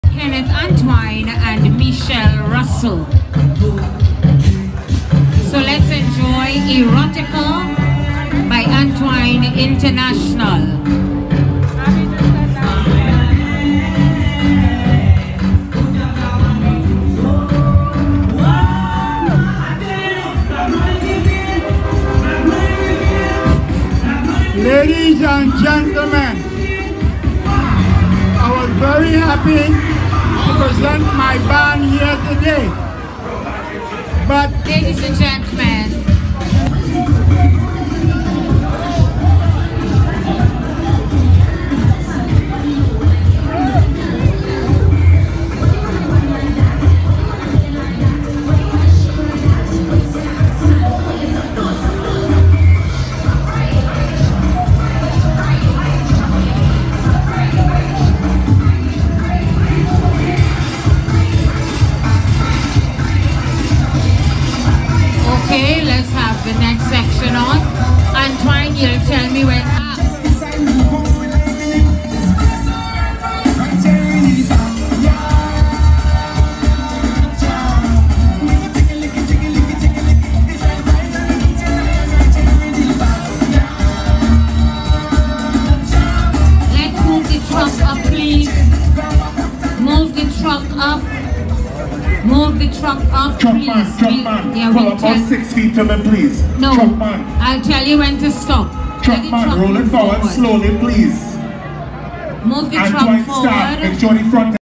Celebration until Sundown # New York City # Carnival # Caribbean # West Indian Festival.
Mas! Mas! Play Mas!

On Labour Day, Masqueraders were showing off their costumes before the judges. They were on the Parkway.  Patrons, including photographers and videographers, flocked to the vicinity, vying for the constrained spaces surrounding the stage.

Hewitt. J
Hewitt's Health, Fitness & Wellness
YouTube.com/@layou05
Instagram.com/hewitt604